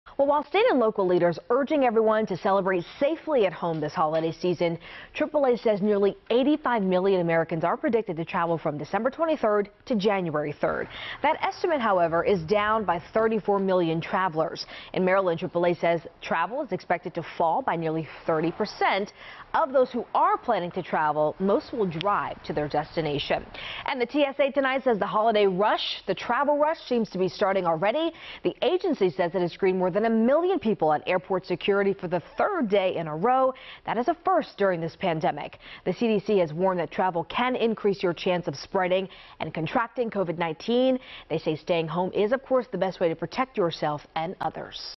AAA Reports 30% Fewer Marylanders Plan To Travel For Christmas, New Year's
The holidays may look a bit different this year, with many people planning to stay home rather than travel to see family or friends as the pandemic shows no signs of letting up through the holiday season.